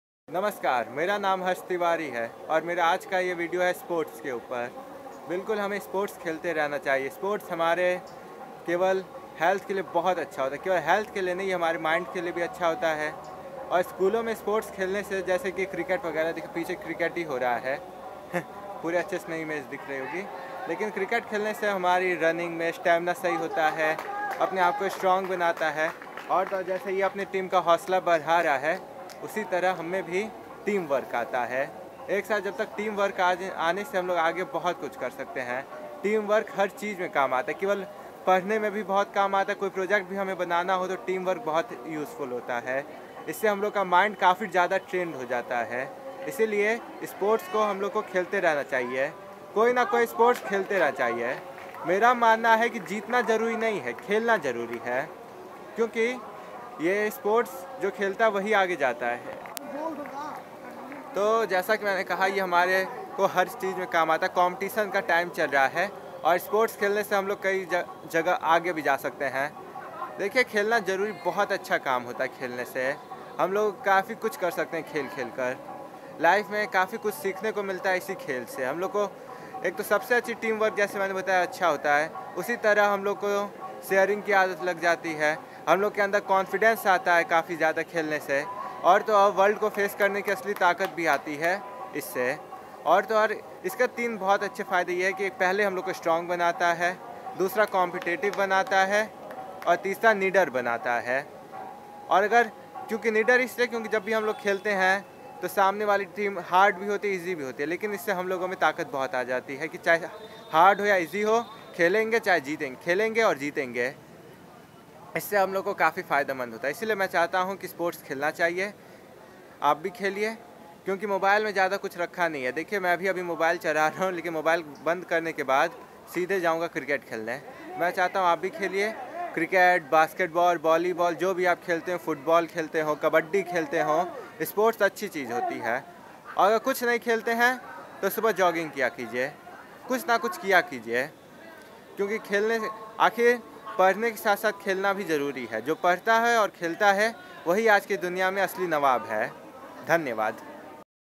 Sports is the New Religion
About that Video

Is video mein Maine sports ka apni jindagi mein kya mahatva h
Tatha issey humein kyaa faayeda milta hai yeh bataaya hai